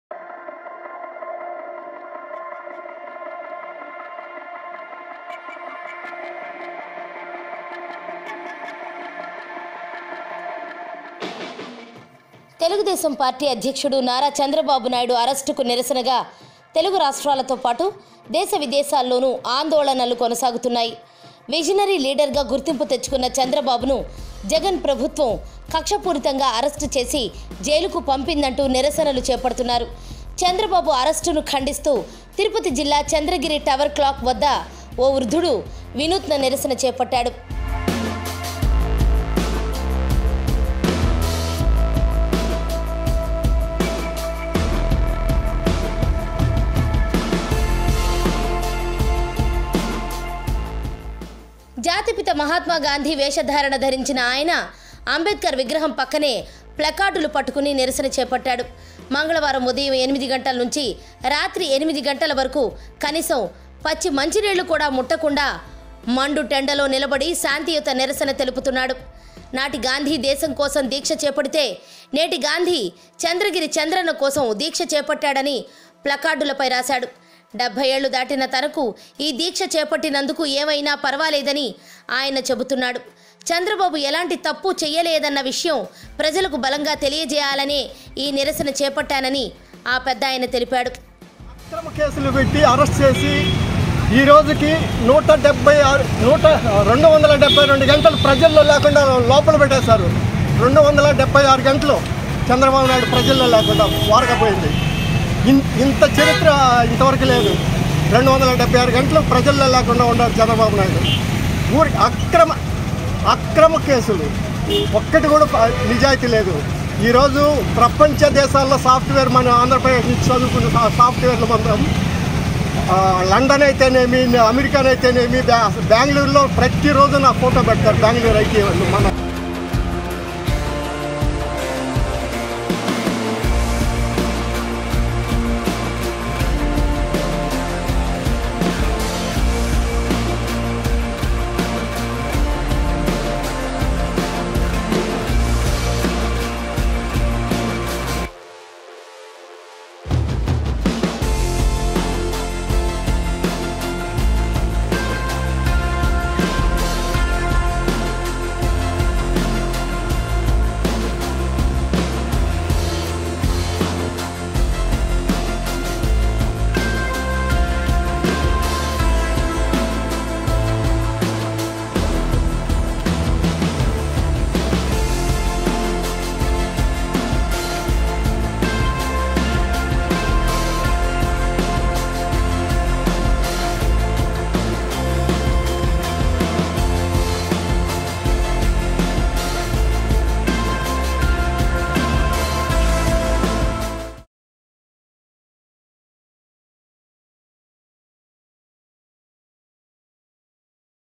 An Old Man Dressed as Mahatma Gandhi Protest on Chandrababu Naidu Arrest | Samayam Telugu
An Old Man Dressed as Mahatma Gandhi Protest on Chandrababu Naidu Arrest.